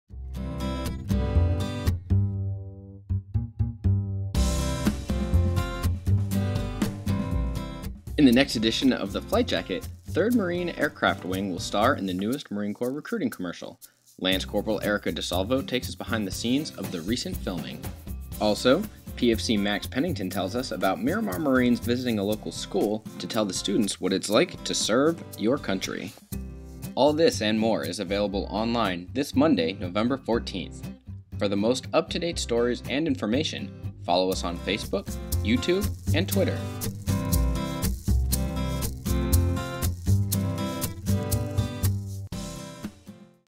Flight Jacket Preview- November 14th Edition
A video preview of the content of November 14th's MCAS Miramar Flight Jacket.